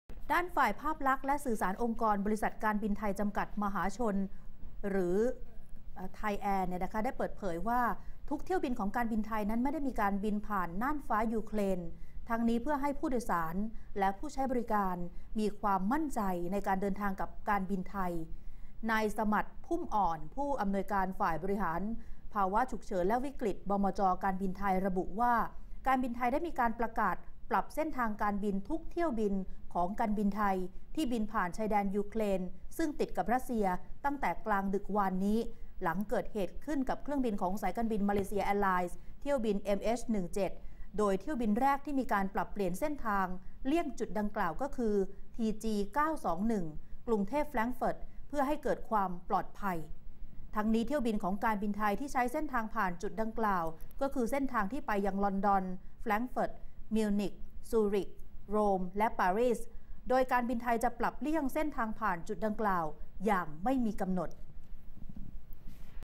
การบินไทยประกาศเลี่ยงน่านฟ้ายูเครน
2014/07/18 การบินไทยประกาศเลี่ยงน่านฟ้ายูเครน

ASTVผู้จัดการออนไลน์ - การบินไทยประกาศทุกเที่ยวบินของการบินไทยเลี่ยงผ่านน่านฟ้ายูเครน หลังจากสายการบินมาเลเซีย แอร์ไลน์ ถูกยิงตก